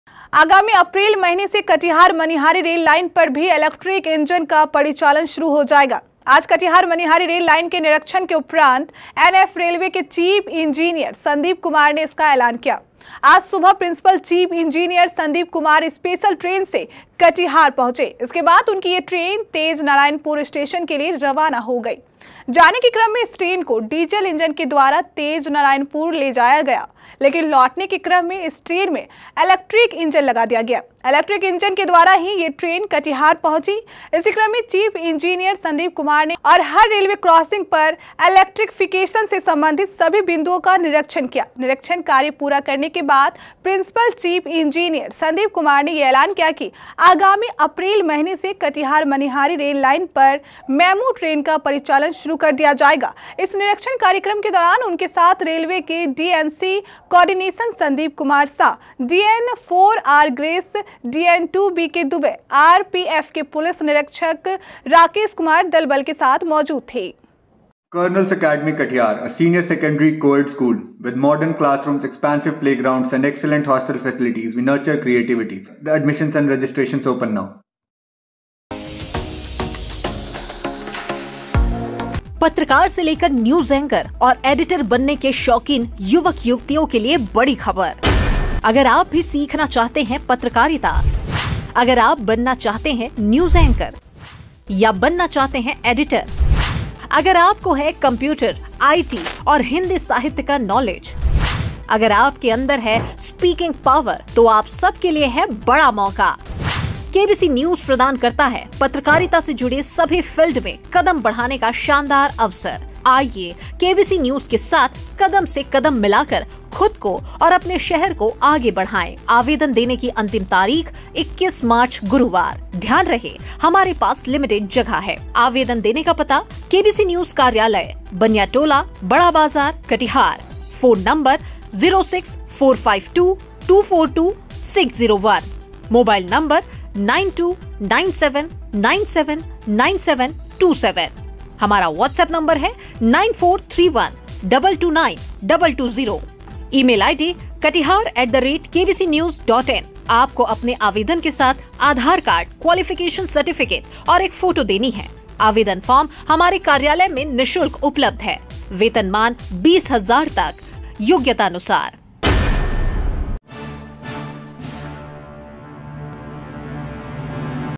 आगामी April से शुरू होगा Manihari Railway Line पर Electric Engine का परिचालन || KBC News
Manihari Railway Line News
Manihari Electric Engine News
Manihari Electric Engine
Electric Engine
Manihari News